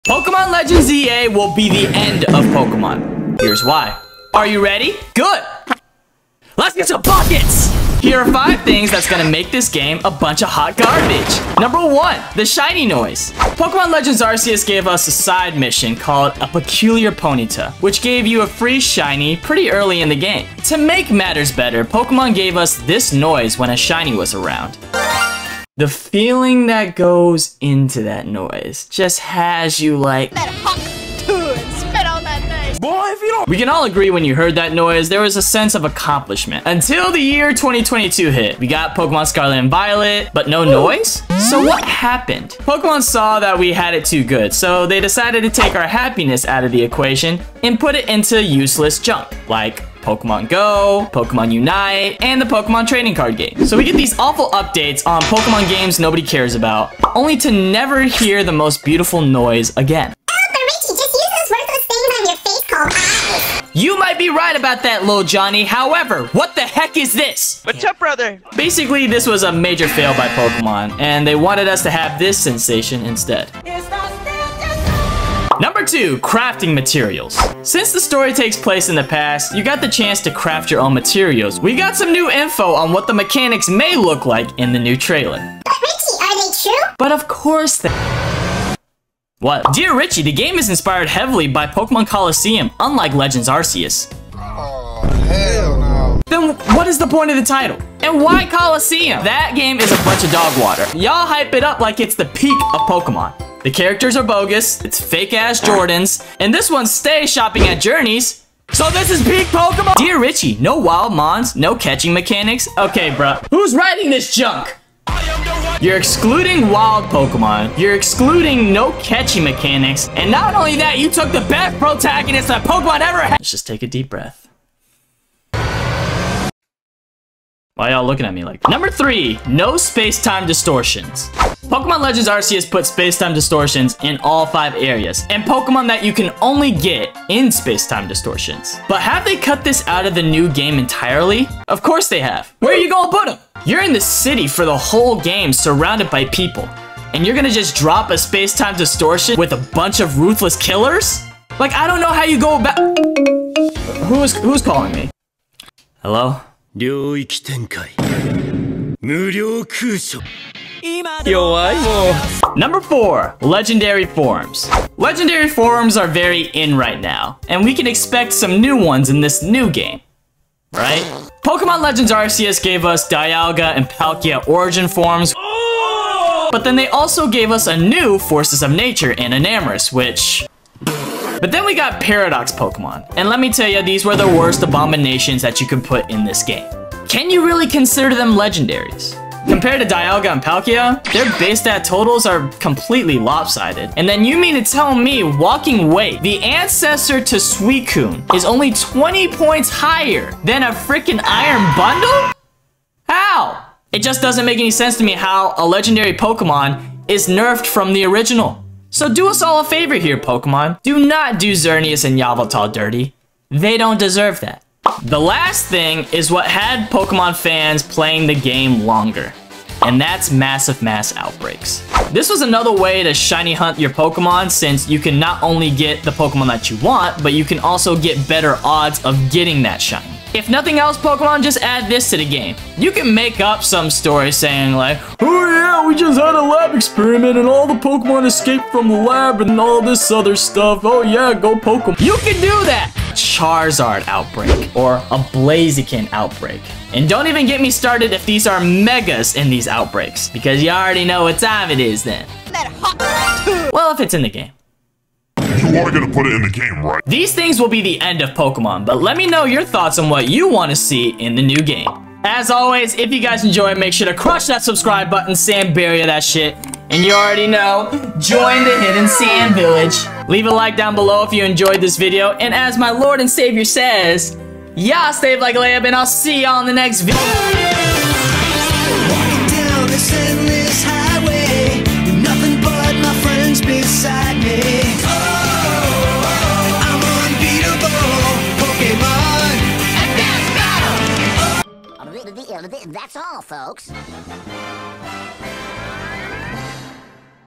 Pokemon Legends Z-A Will Be THE END of Pokemon… Here’s Why
Pokemon Legends Z-A Will Be THE END of Pokemon… Here’s Why. In this video, we dive deep into the latest Pokemon news and leaks surrounding the highly anticipated game, Pokemon Legends Z-A. Join us as we discuss why Pokemon Legends Z-A could potentially mark the end of the Pokemon series as we know it. From new Pokemon leaks to the exciting possibilities with the Legends Z-A starters, we cover it all.

Edited by:  @sofrun
Thumbnail:  @brushandbeyond